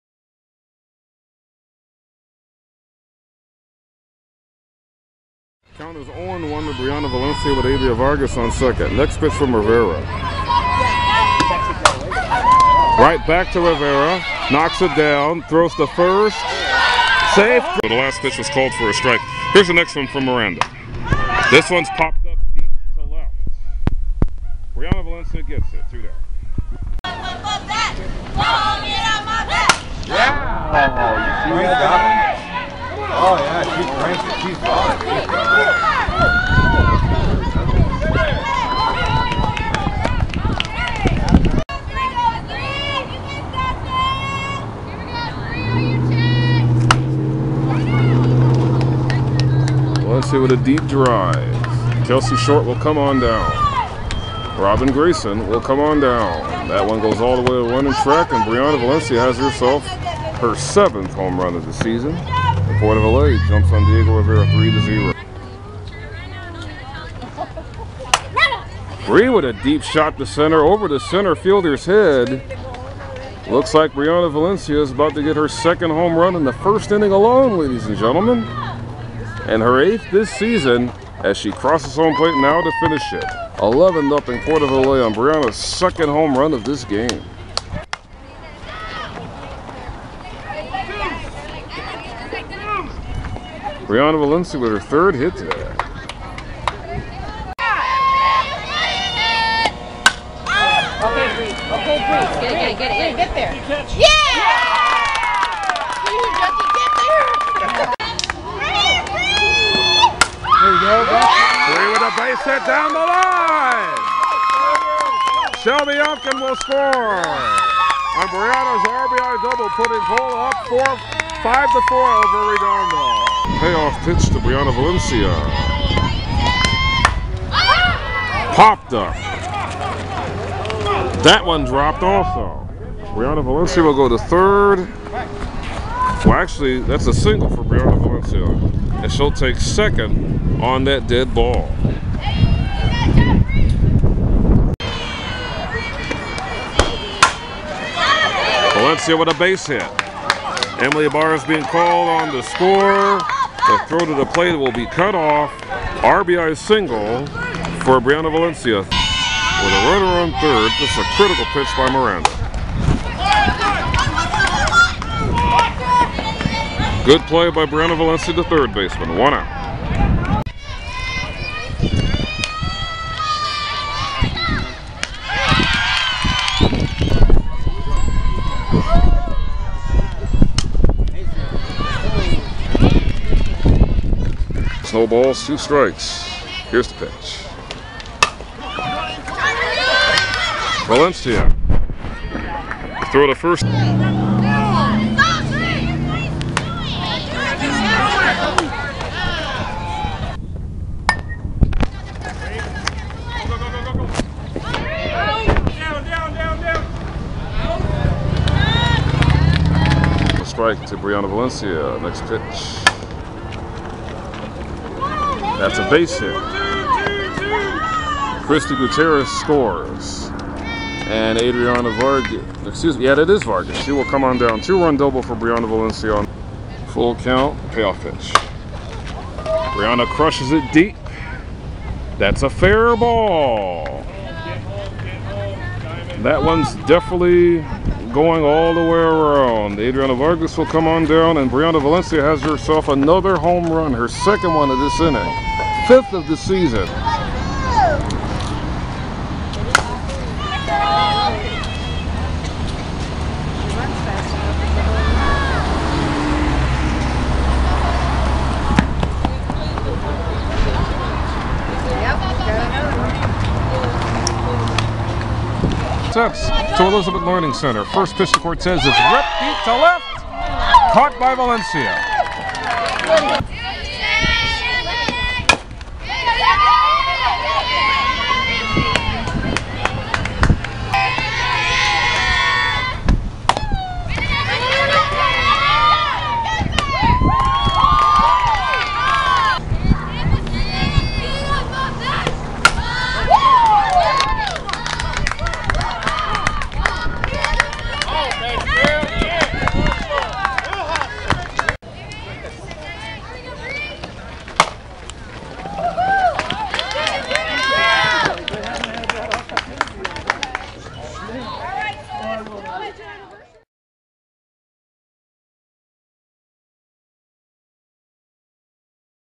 Briana Valencia Highlights: 2014 Port Of Los Angeles High Softball
A montage of highlights from Briana Valencia during the 2014 season for Port Of Los Angeles High School softball.